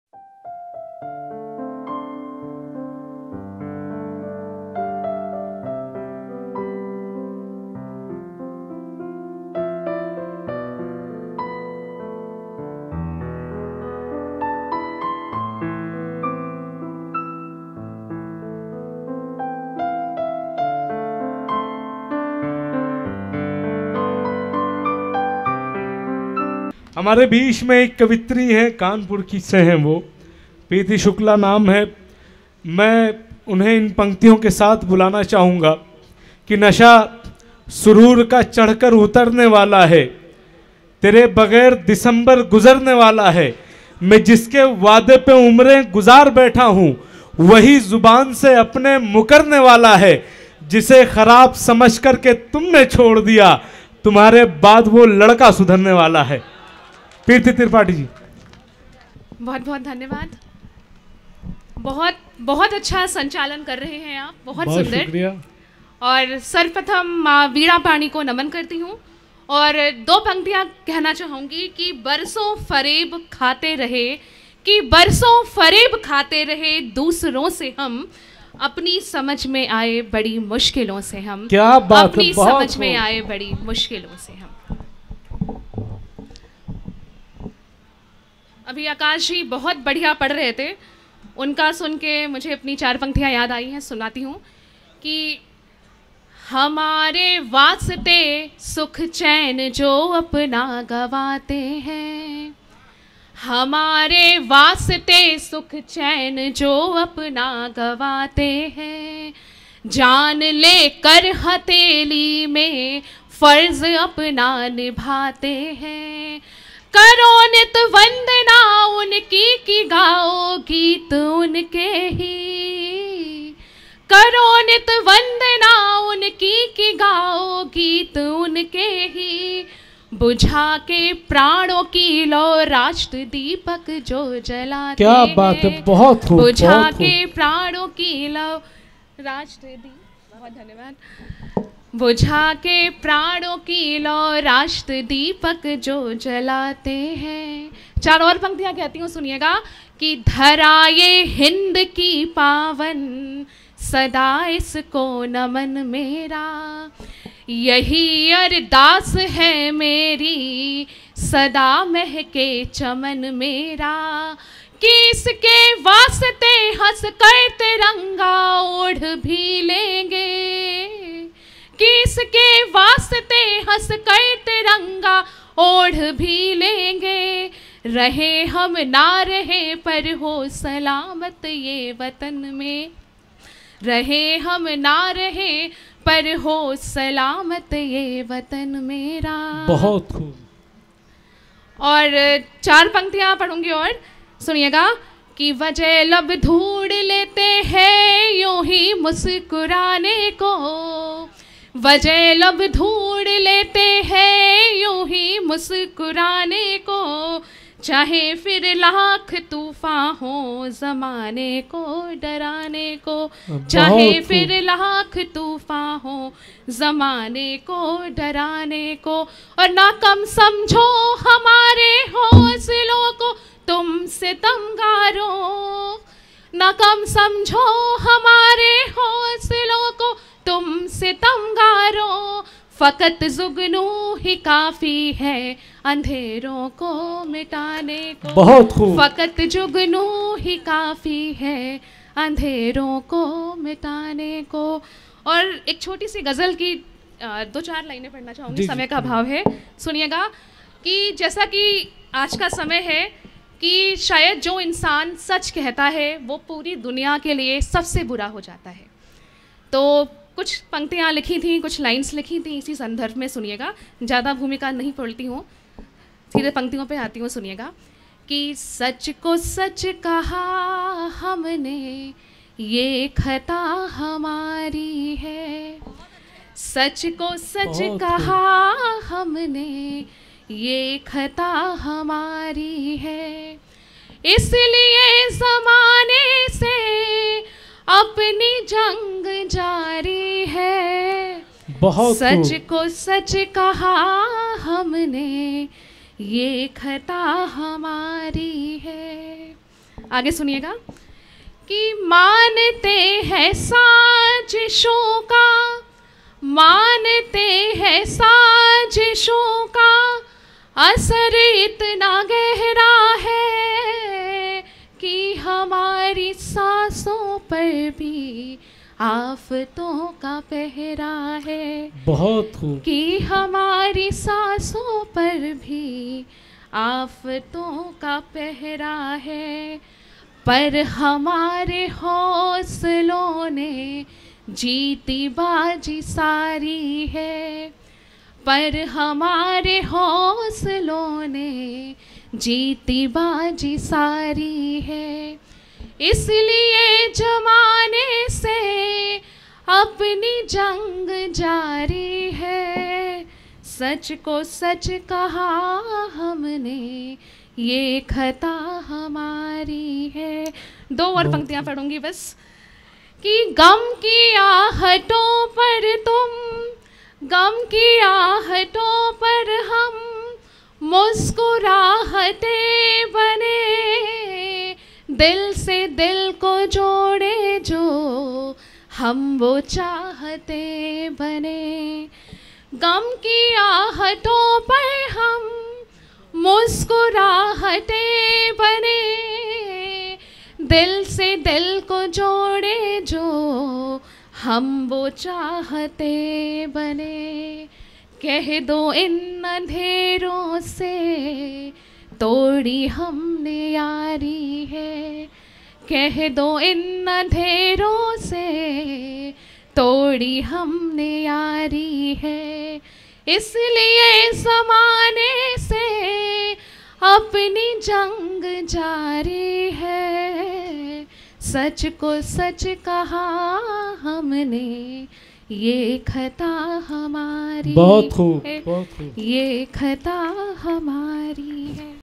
Priti Shukla | Akhil Bhartiya Kavi Sammelan Mushaira | Avadh Shilpgram | Lucknow | 2023
Priti Shukla |
Akhil Bhartiya Kavi Sammelan Mushaira |
Avadh Shilpgram | Lucknow |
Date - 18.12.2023
Mobile No.

Head Office.

Branch Office.